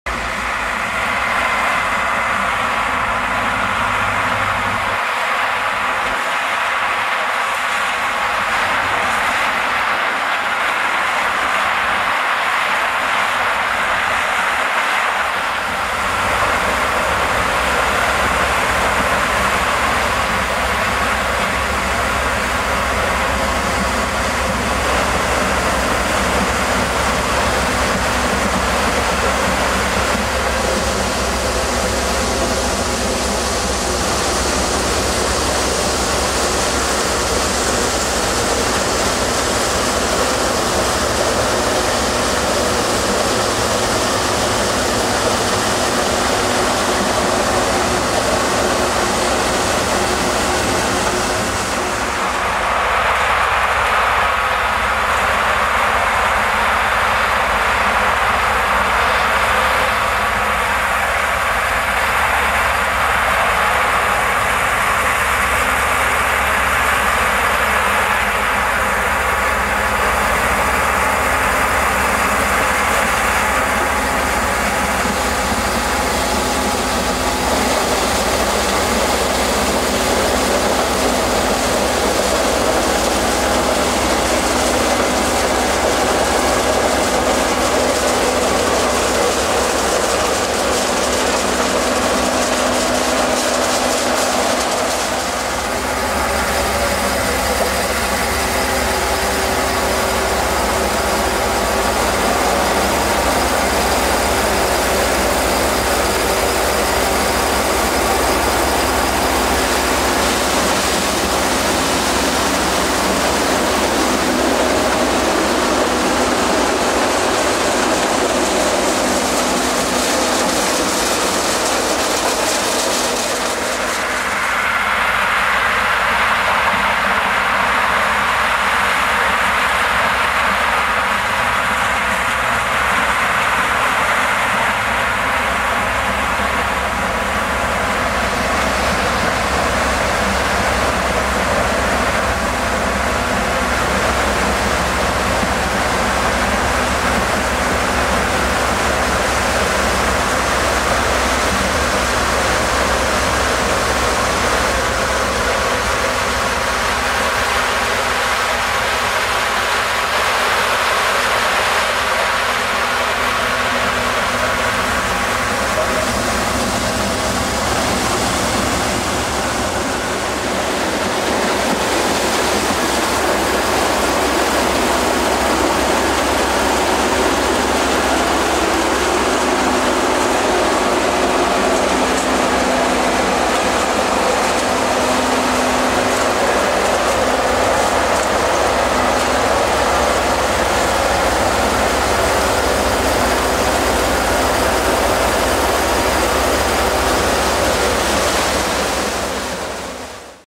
Maïs 2011 | Maïsdorsen | Fendt Xylon | Case International 1620 met 6 rijer Geringhoff plukker
Deurne, 17-10-'11
Bennenbroek uit Veulen
Case International 1620 met 6 rijer Geringhoff plukker
Fendt Xylon met kipper